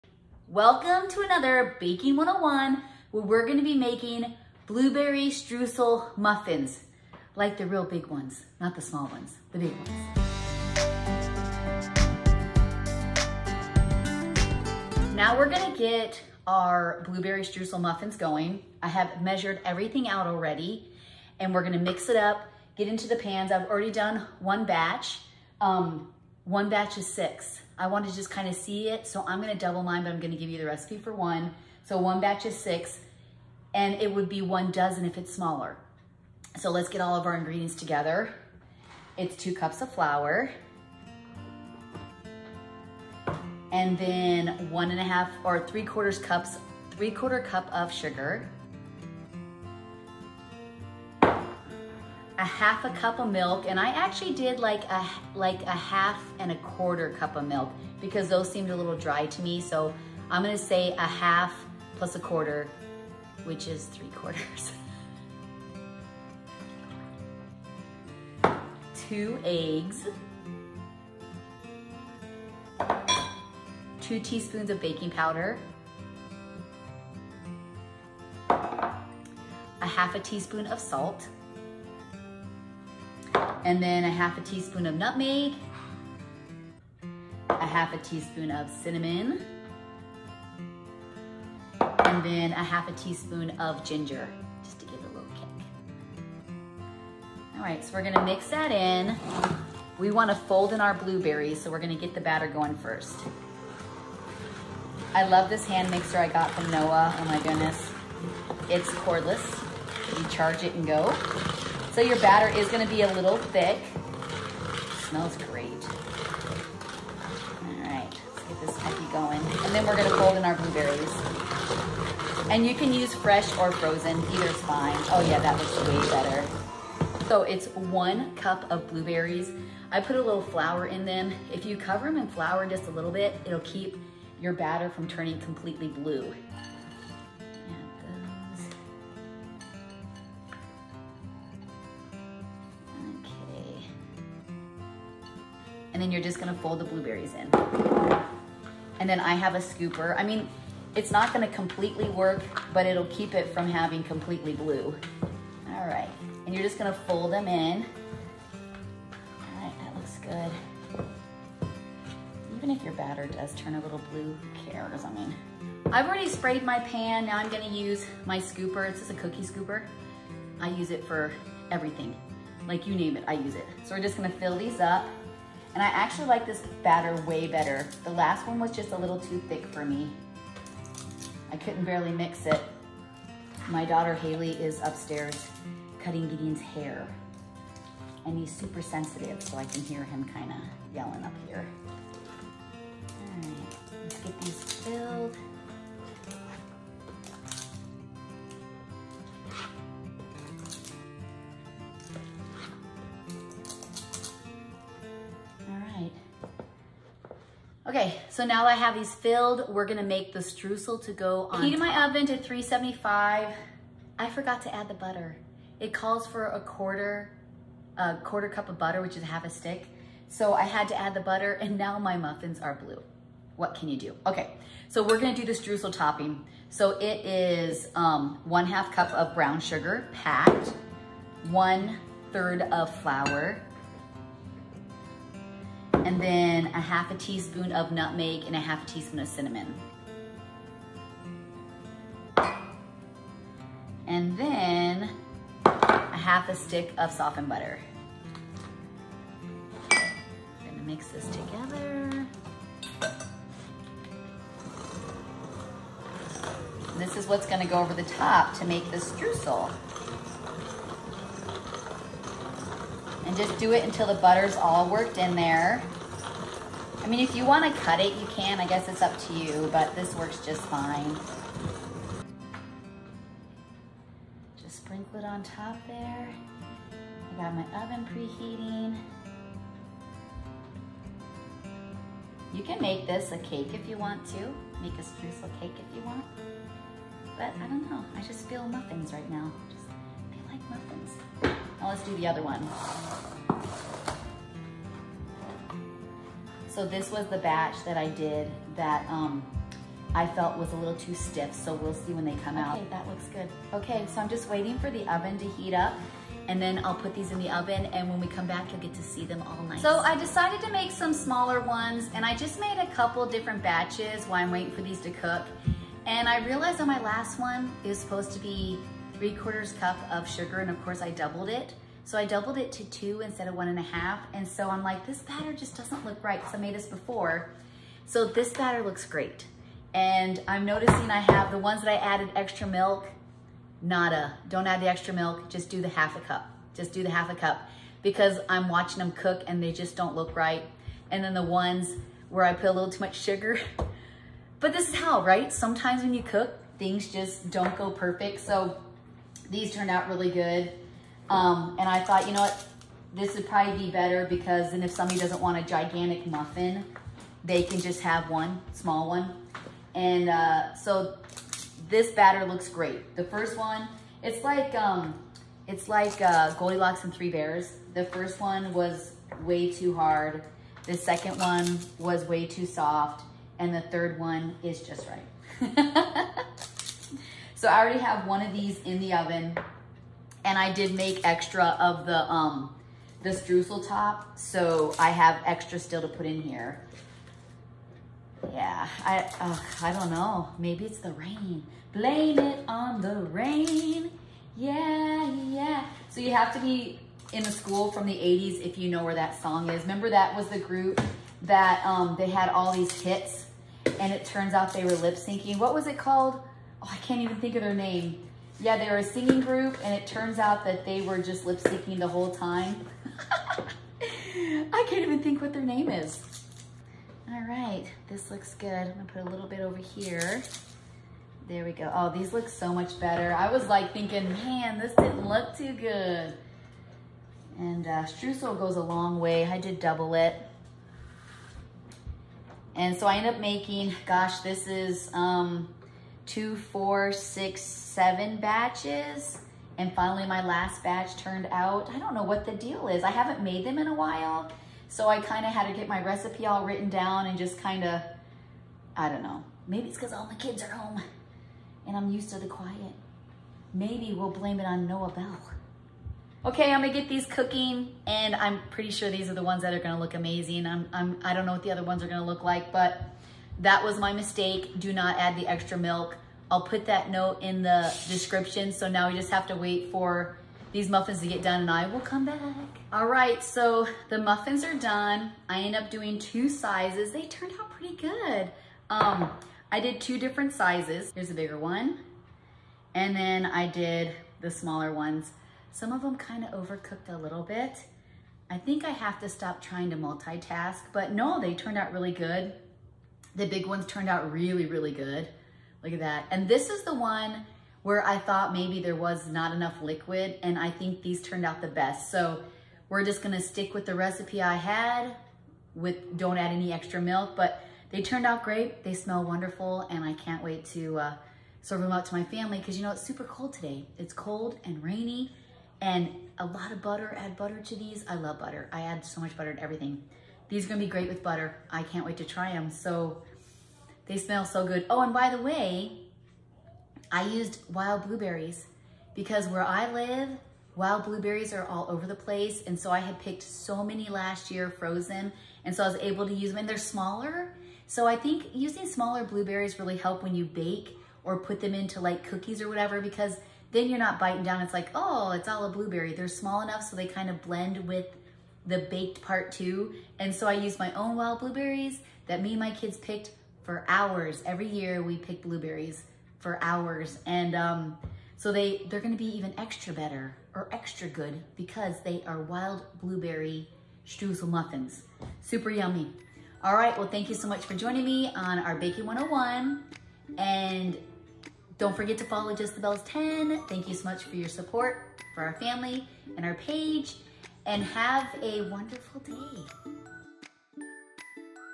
BAKiNG 101~ SUPER EASY BLUEBERRY STREUSEL MUFFiNS 😋☕️
These blueberry, streusel muffins are amazing. You can also just make it into a cake if you’d like. It taste just as great.  I also have a popular, blueberry muffin that taste just like the good old days when we were young. ￼ I remember my mom making these when I was a kid and they’re still just as good as when she made them. But for now here’s the recipe for the streusel muffins

Blueberry Streusel Muffins Recipe:

Batter Ingredients:

2 cups of flour
3/4 cup of sugar
1/2 cup of whole milk
1/4 cup of softened butter
One large egg
2 teaspoons of baking powder
1/2 teaspoon of salt
1/2 teaspoon of cinnamon
1/2 teaspoon of nutmeg
1/2 teaspoon of ginger
One cup of fresh blueberries

Streusel Topping Ingredients:

1/2 cup of brown sugar
1/3 cup of flour
1/2 teaspoon of nutmeg
1/2 teaspoon of cinnamon
1/4 cup of soften butter

Directions:

Combine all the ingredients, except for the blueberries, and mix with your mixer until it’s well blended.
Then fold in your blueberries, and using a scooper put into each of your cupcake liners or oiled cupcake pan.
Next, make your streusel by combining all of your ingredients, using a blender to mix it together.
Sprinkle over the top of your blueberry muffin batter in the muffin pan.
Bake at 375° for about 15 to 20 minutes
and checking them by putting a butter, knife or fork into the middle and if it comes out clean they are done. ￼

thank you for being a part of another cooking 101. ￼